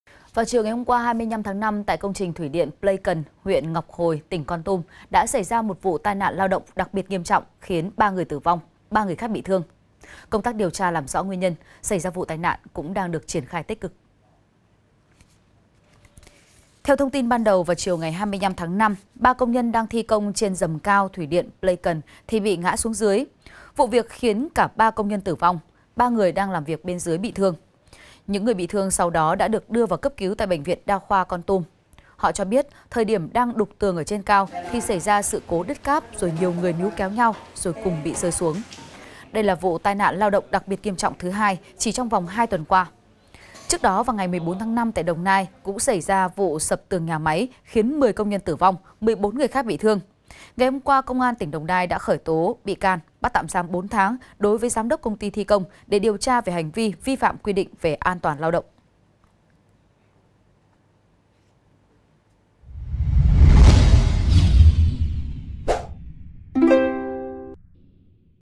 3 công nhân tử vong tại công trình thủy điện: Do đứt cáp? | VTC14
VTC14 | 3 CÔNG NHÂN TỬ VONG TẠI CÔNG TRÌNH THỦY ĐIỆN: DO ĐỨT CÁP?
Vào chiều qua 25/5, tại công trình Thuỷ điện Plei Kần (huyện Ngọc Hồi, tỉnh Kon Tum) đã xảy ra vụ tai nạn lao động đặc biệt nghiêm trọng khiến 3 người tử vong, 3 người khác bị thương. Công tác điều tra, làm rõ nguyên nhân xảy ra vụ tai nạn cũng đang được triển khai tích cực.